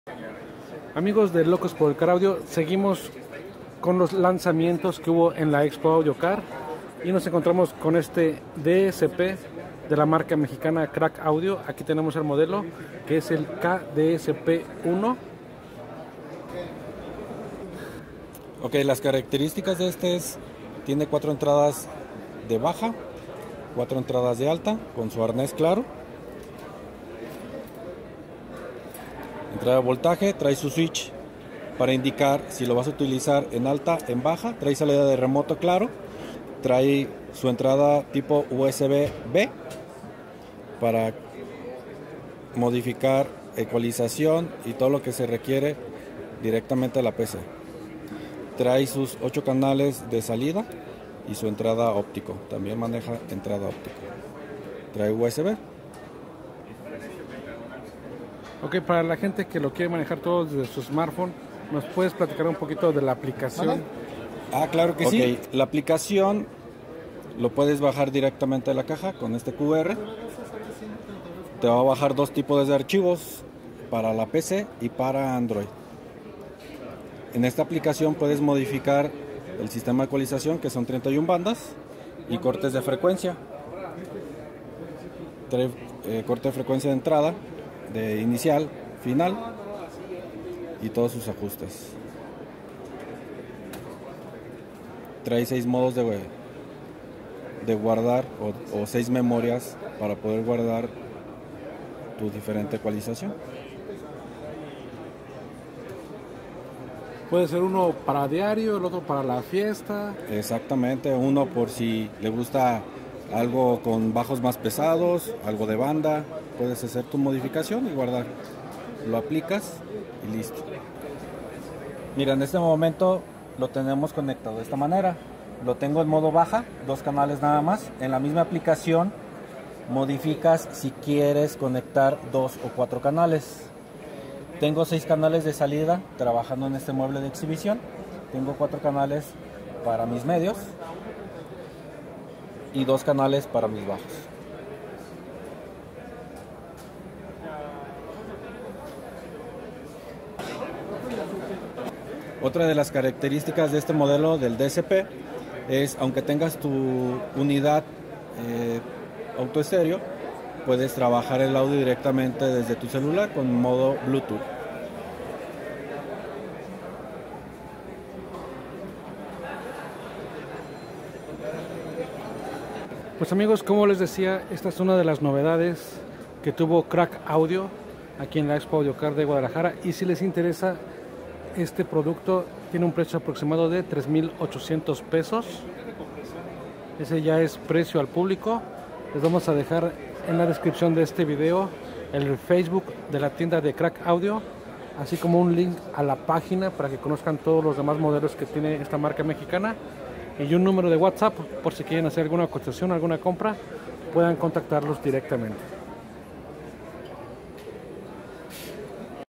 ¡Nuevo DSP Krack Audio en 3800 pesos!
Fue uno de los lanzamientos de esta marca mexicana en la ExpoAudioCarGdl